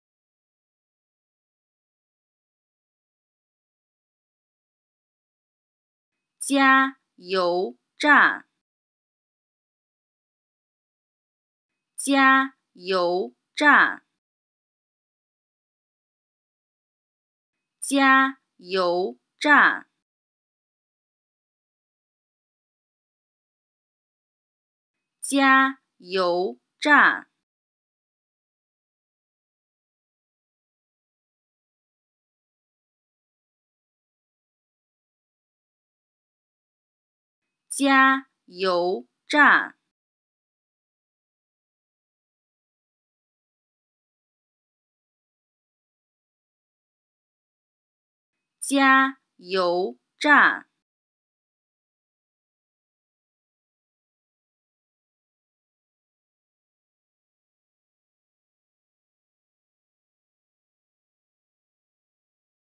How To Say ''Gas station'' (加油站) in Mandarin Chinese
Learn how to pronounce ''Gas'' (煤气) in Mandarin Chinese with this short pronunciation video